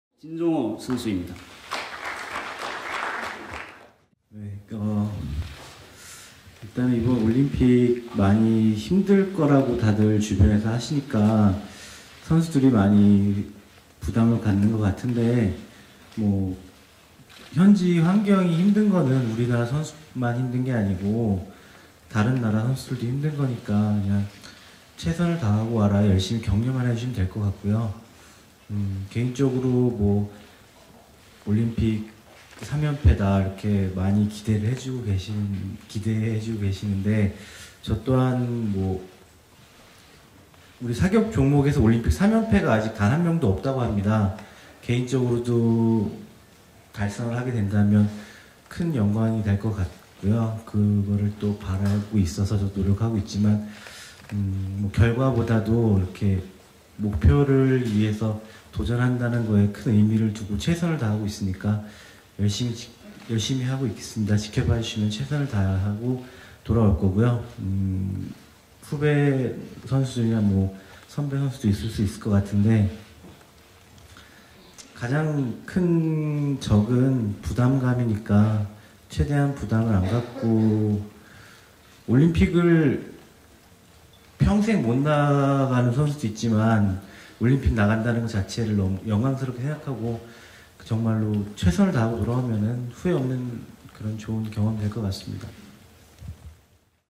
사격 진종오에게 리우올림픽이 갖는 의미
'사격 황제' 진종오가 2016 리우올림픽에서 한국 사격 최초로 3연패에 도전한다.